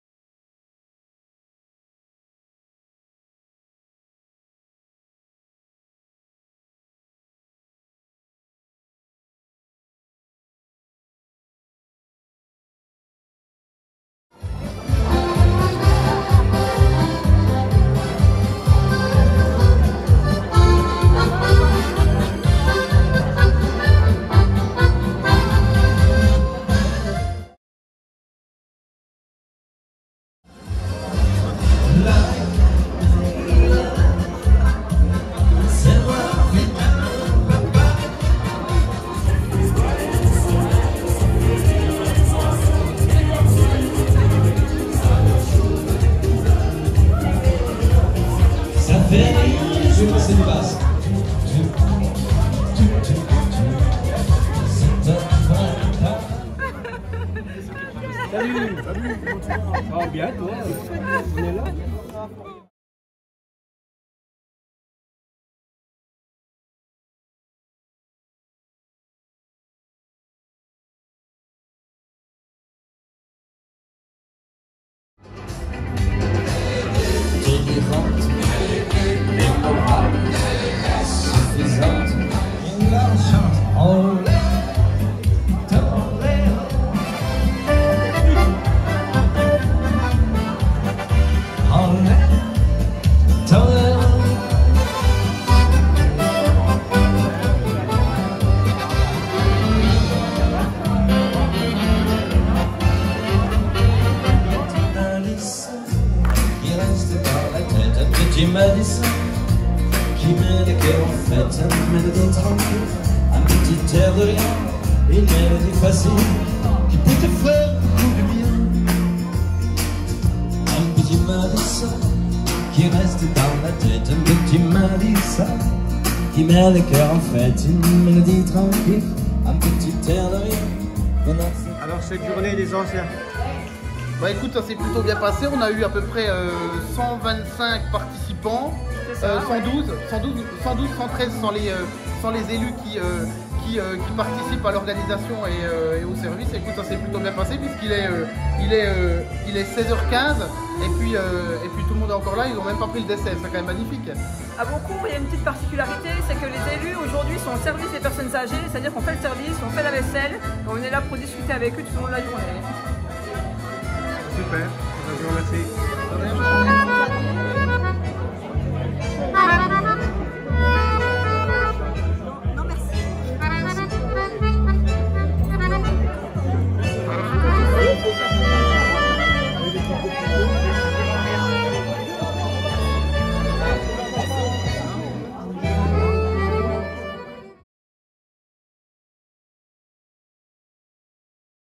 Repas des anciens à Beaucourt
le repas des Anciens s'est deroulé le  Beaucourt au foyer Georges Brassens